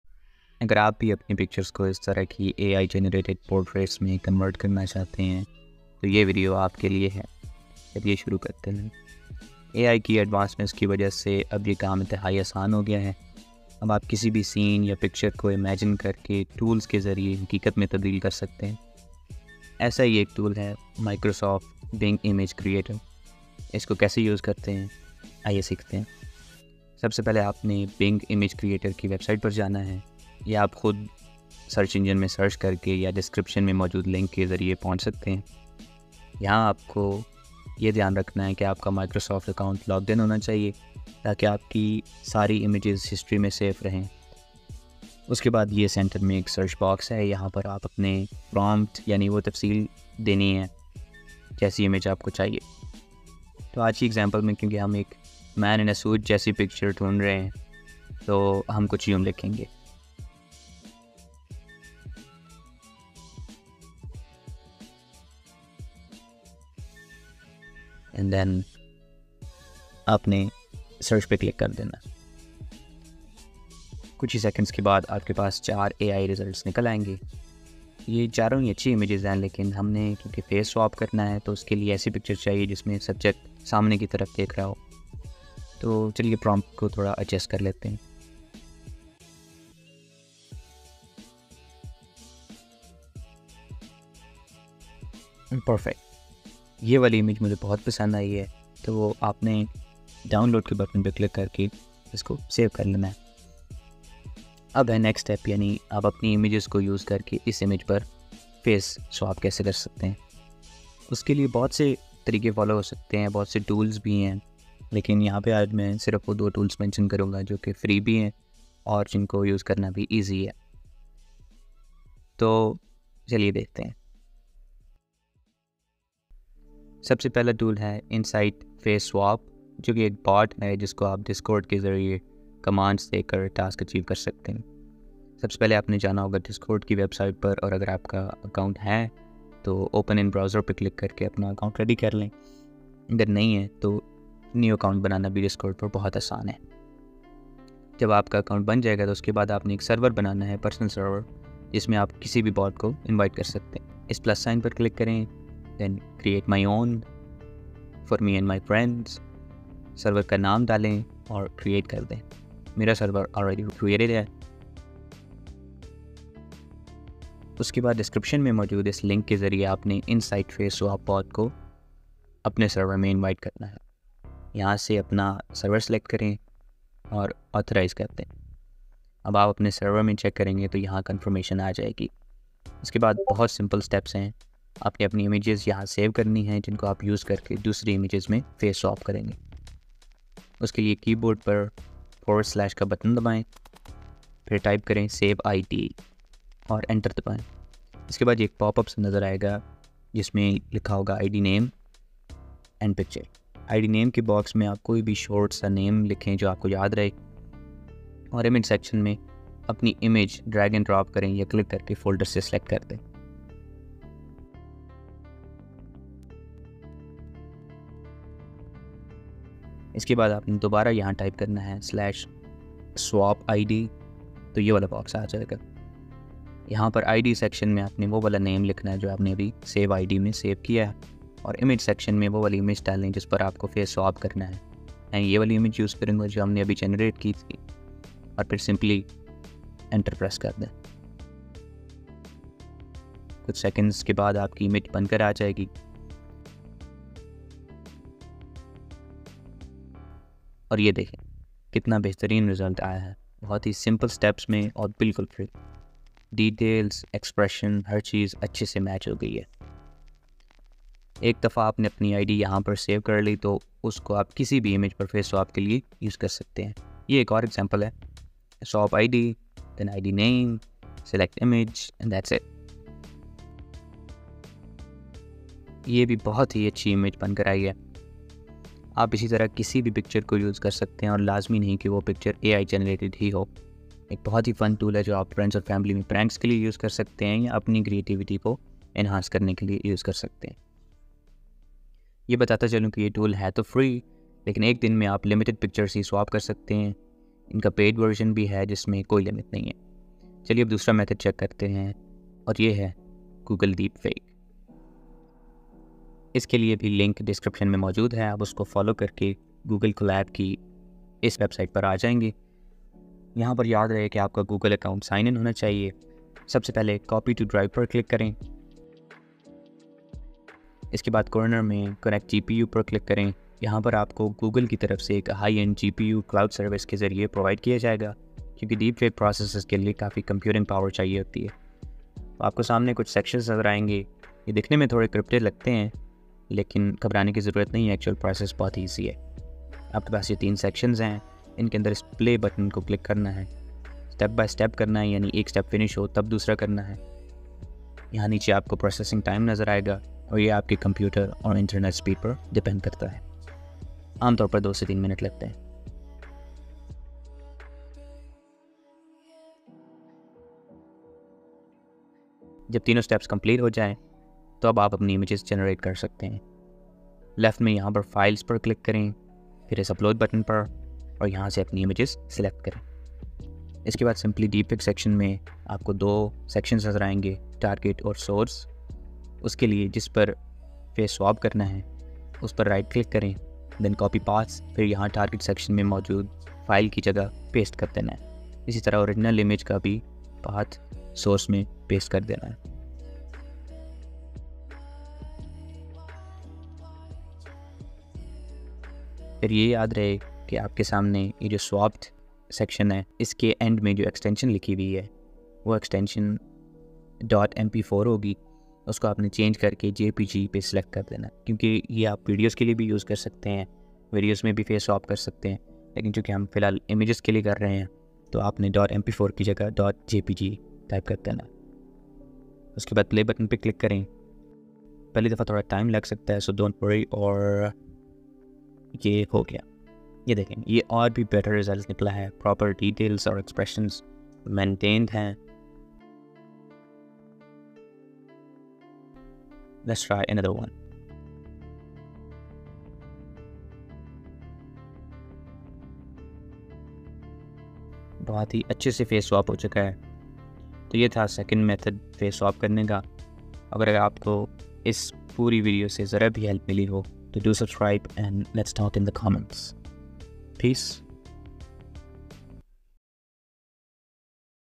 Generate Personal AI Portraits for FREE - High Quality AI Self Portraits
This is how you can use easily available tools to make your own Ai portraits. With your face on it.

Shukria.